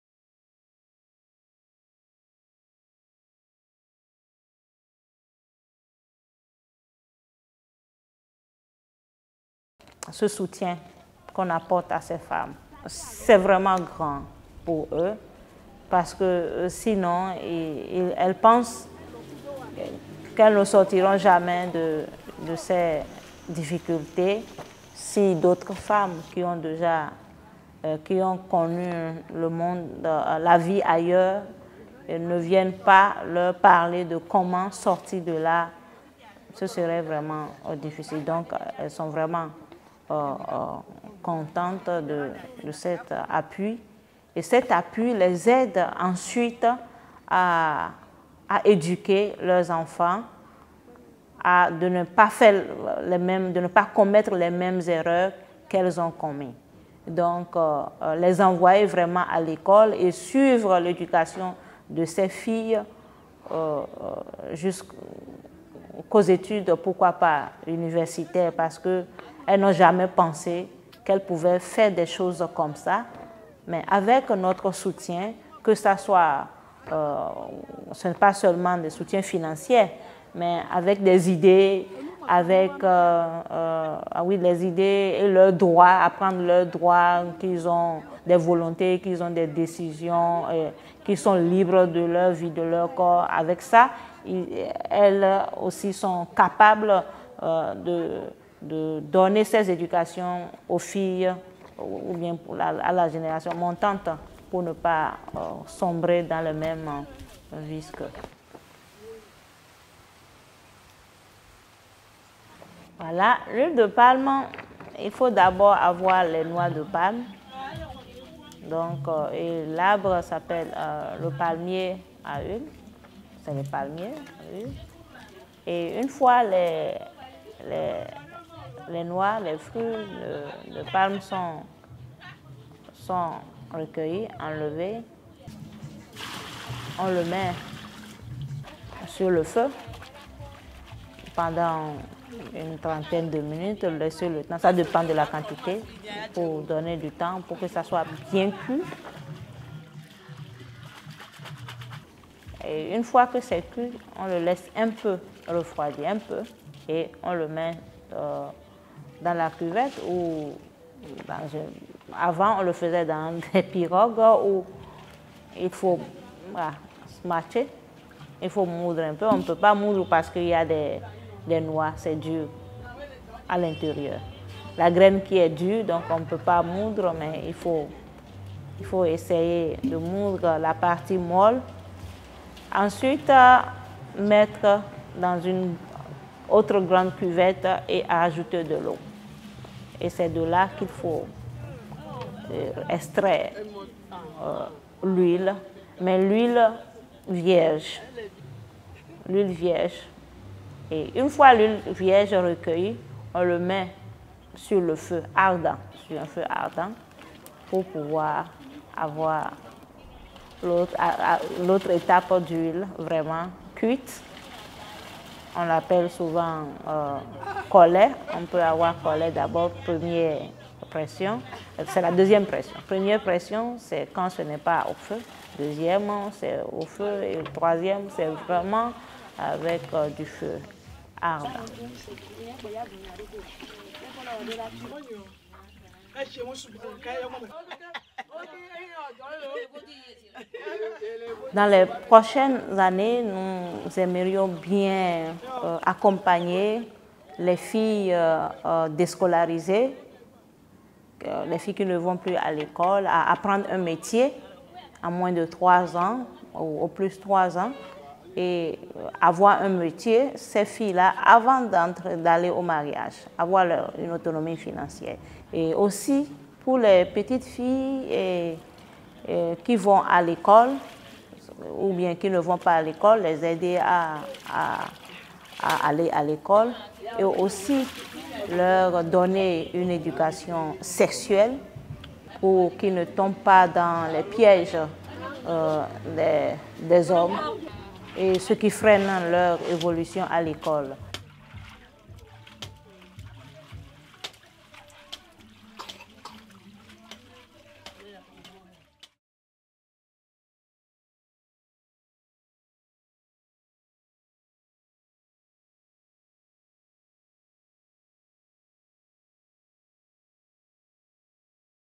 Gnonnou Tchité - Association de femmes au Bénin
Le soutien de l'association & Production d'huile de palme